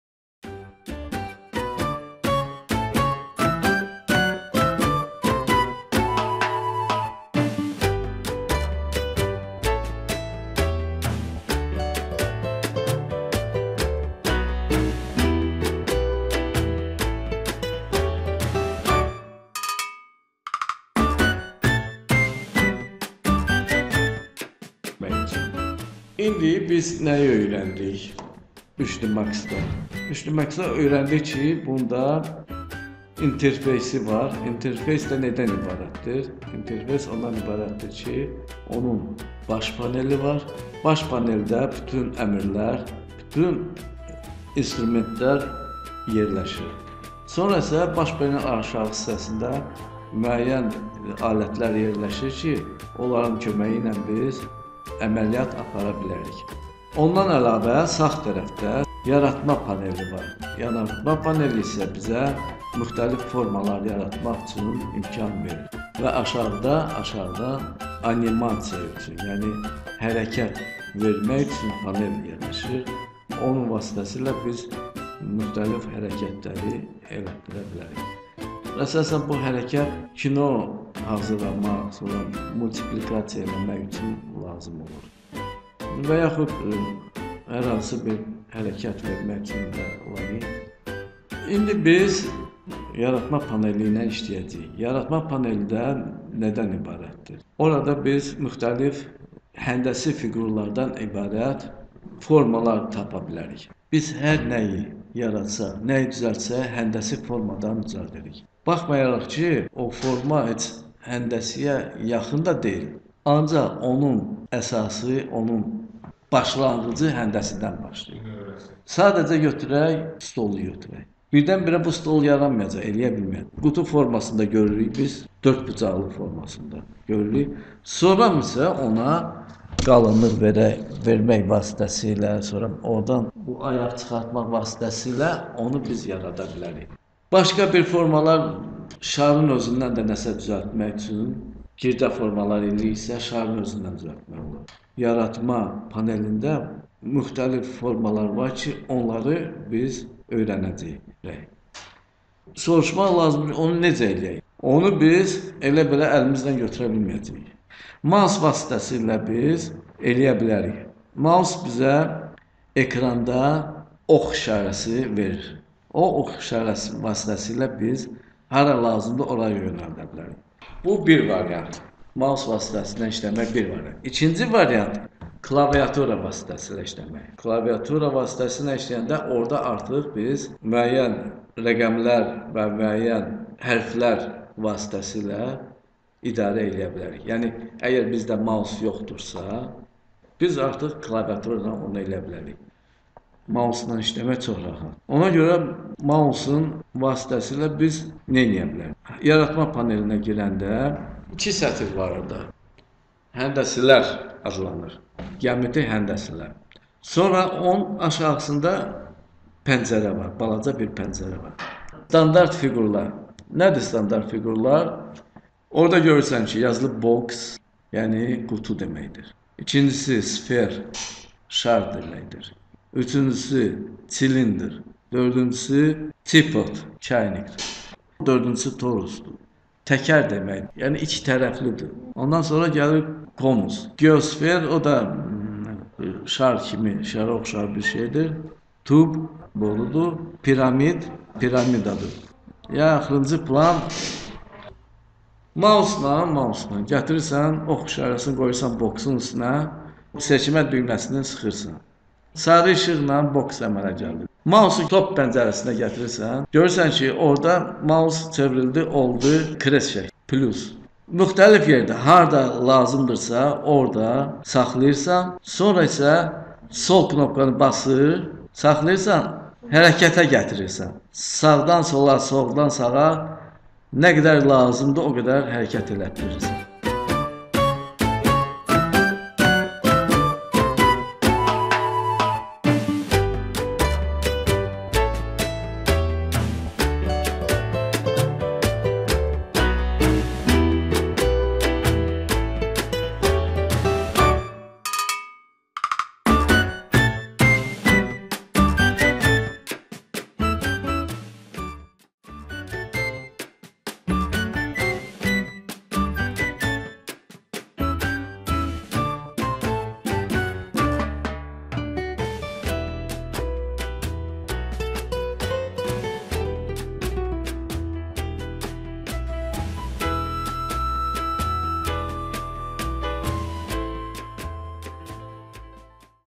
3d max interface 3
3D max - трёхмерный графический редактор для работы на компьютере начальный курс
3D max - kompüterdə işləmək üçün üç ölçülü qrafik proqramdır başlahğıc